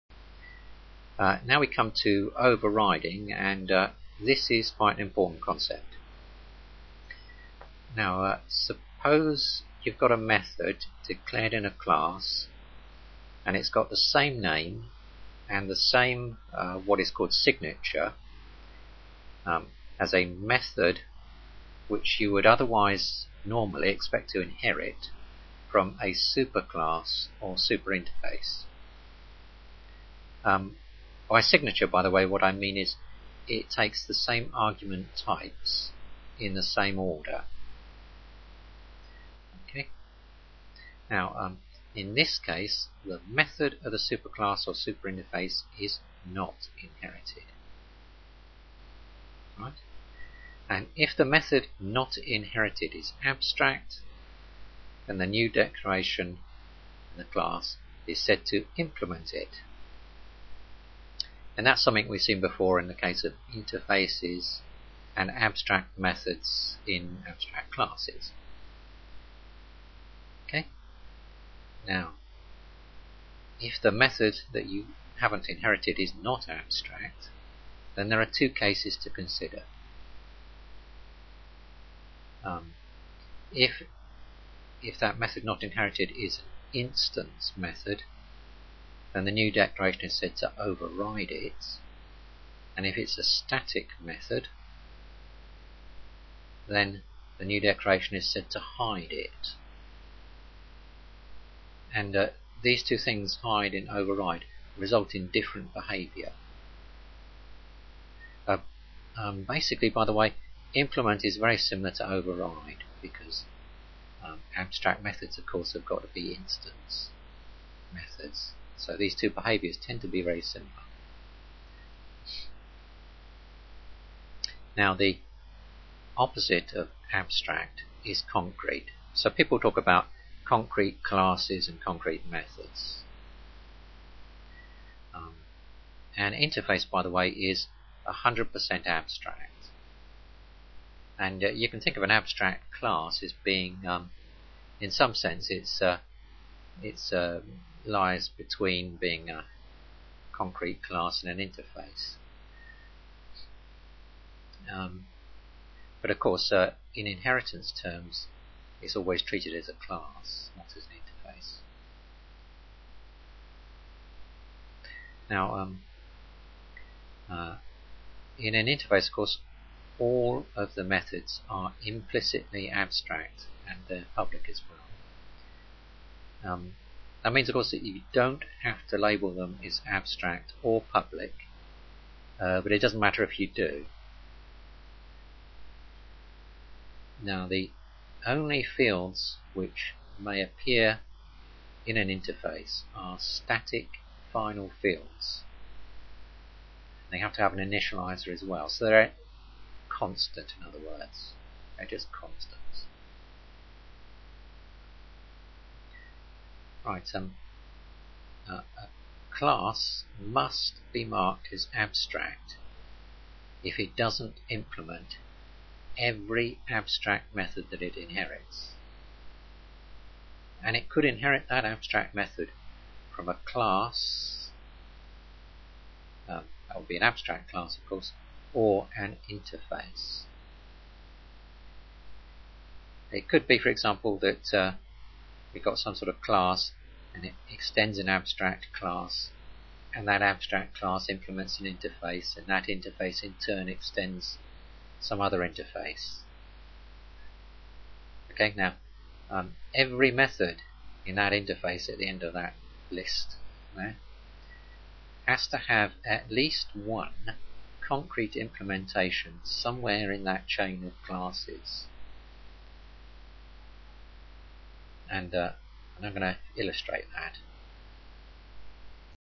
Java Tutorial 17.04 Classes, overriding, hiding and access
A second look at classes: overriding/hiding methods and fields. Confused about overriding? Don't understand the difference between override and hide? Then this is for you. Over 2 hours 32 mins long! This covers access qualifiers, overriding, hiding and implementing in classes, interfaces and abstract classes.